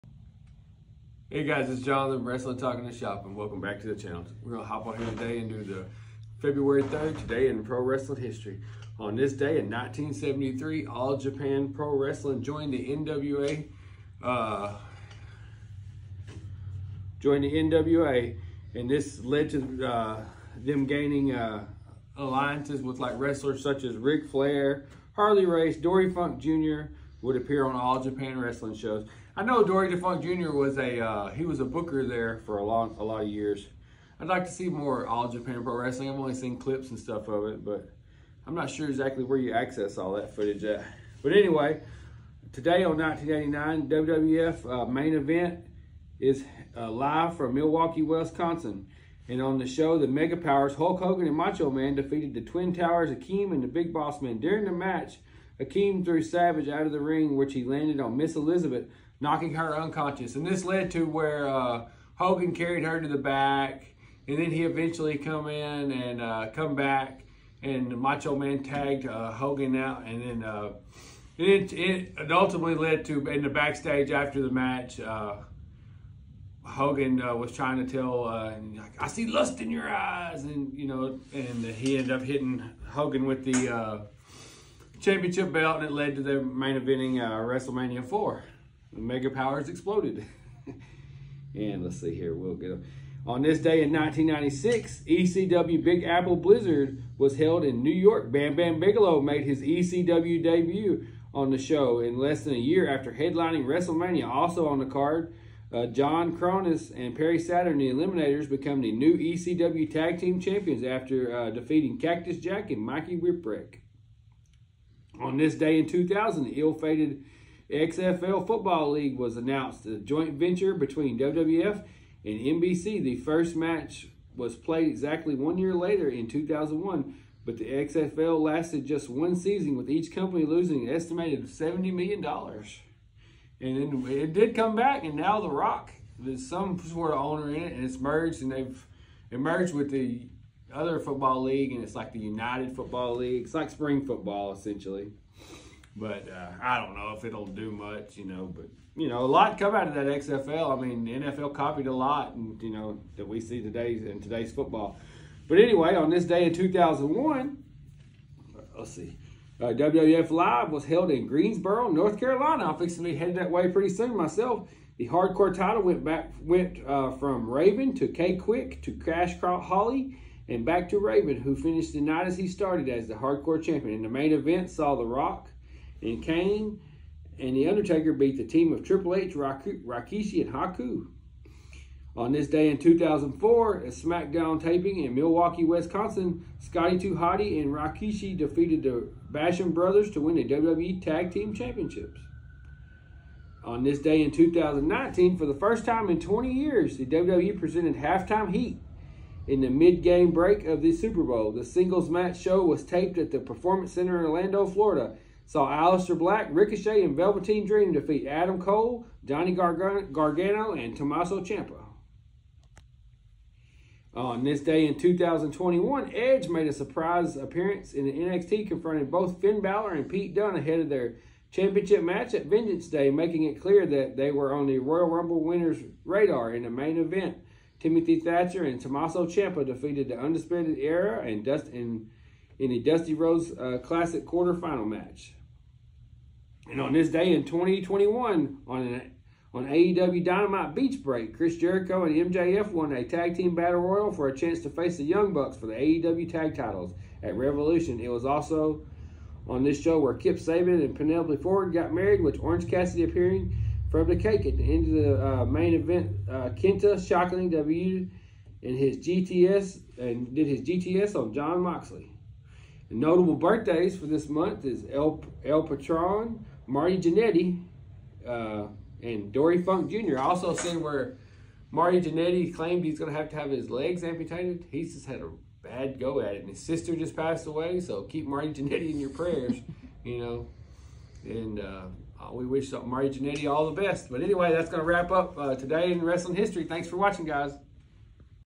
Today in Professional Wrestling History 2/3/24 Mega Powers Implode & more. WWE WWF AEW
Today in Professional wrestling history. 2/3/24. Mega powers Implode & more